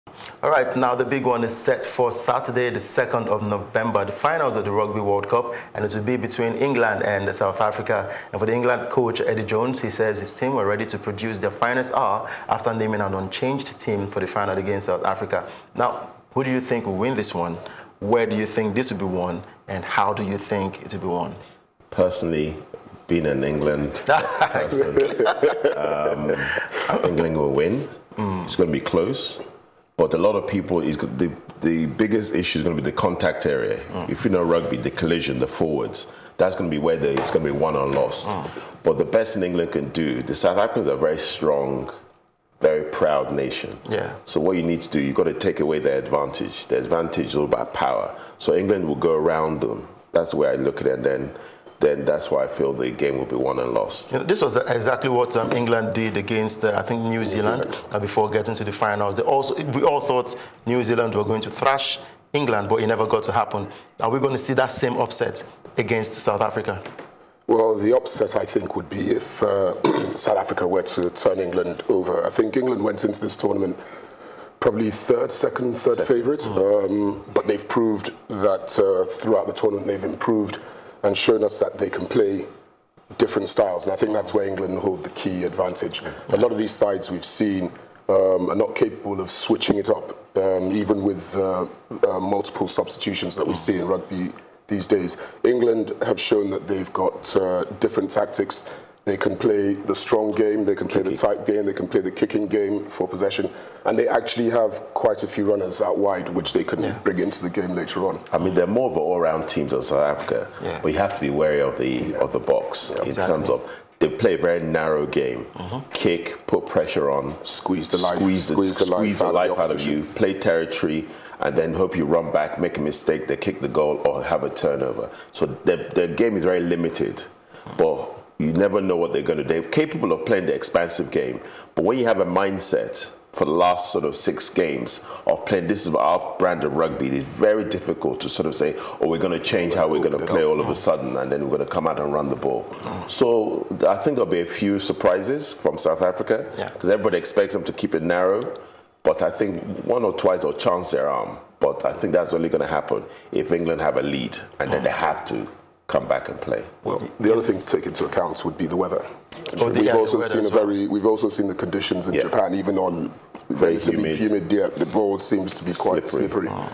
PLUS SPORTS: Analysing the final of the Rugby World Cup.
Will England continue their impressive run or will South Africa stop them and lift the title?